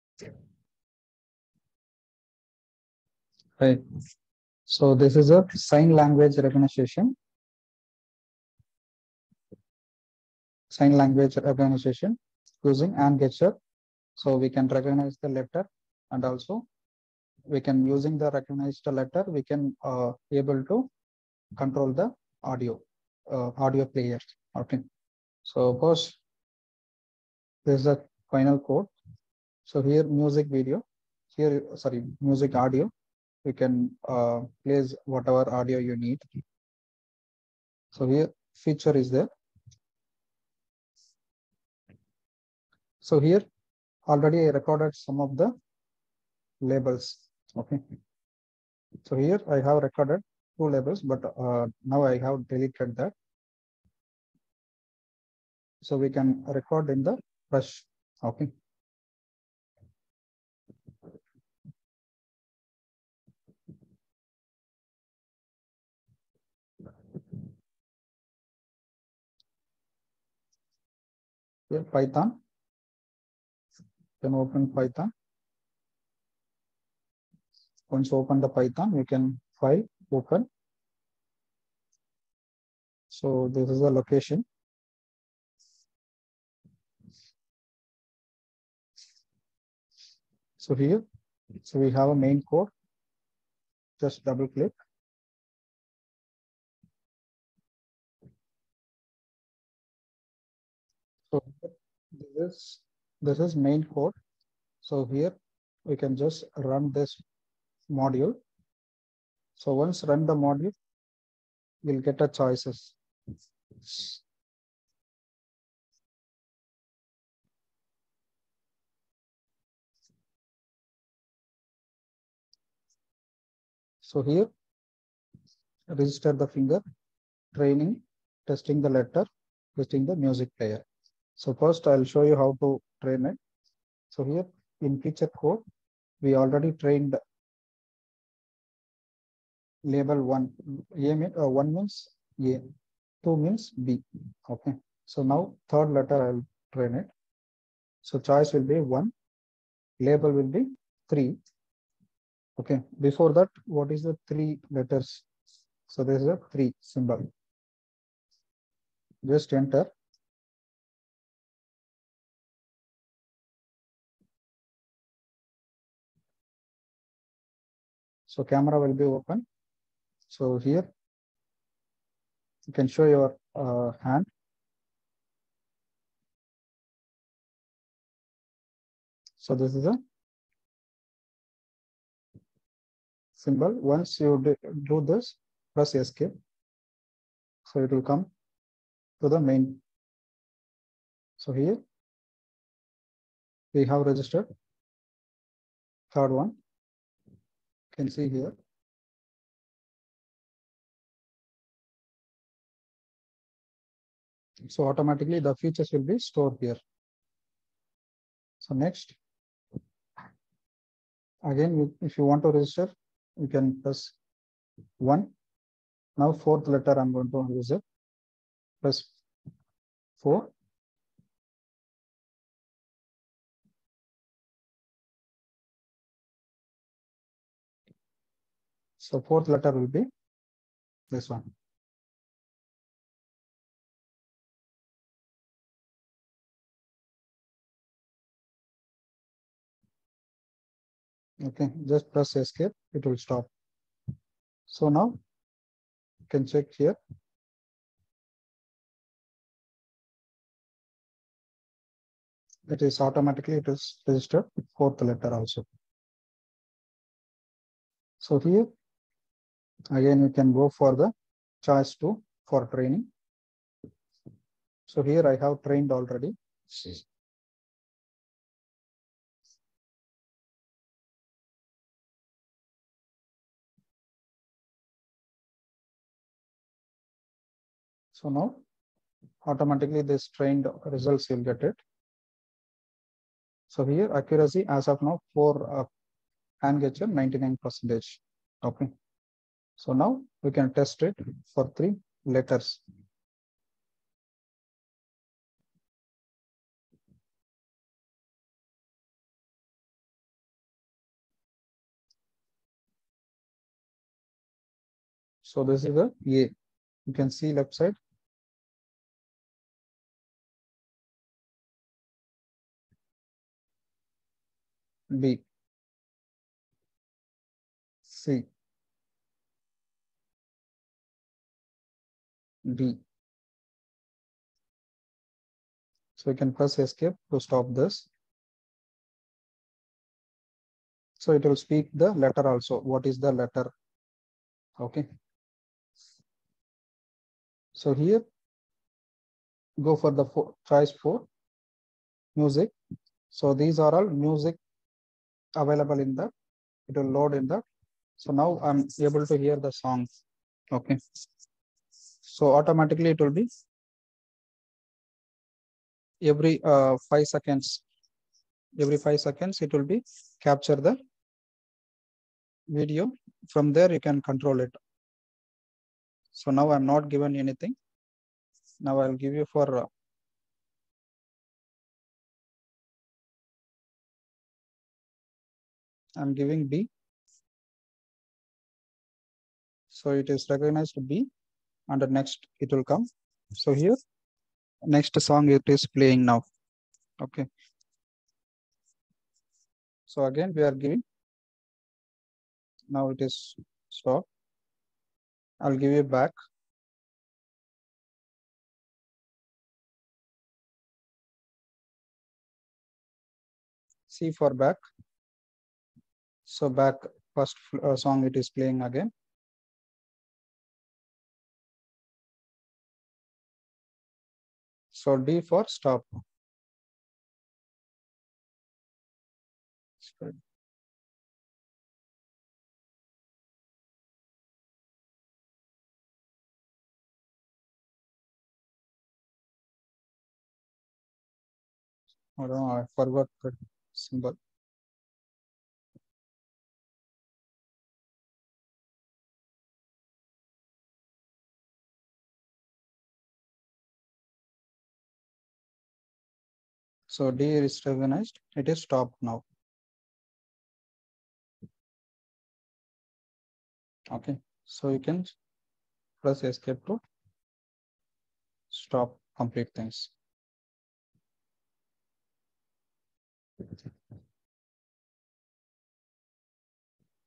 Real time Music player control using hand gesture recognition with deep learning algorithm
Aislyn Technologies Pvt Ltd || Engineering Projects || Projects Consultants || IEEE Projects || final year projects || Mtech Projects || Best Project centre in Bangalore.